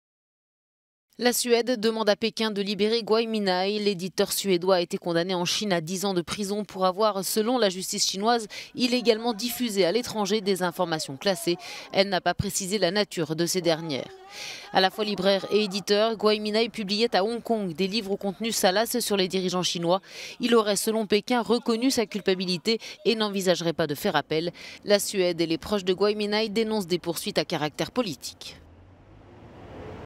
La Suède demande à Pékin de libérer l'éditeur chinois Gui Minhai
Gui Minhai avait été condamné lundi en Chine à dix ans d'emprisonnement.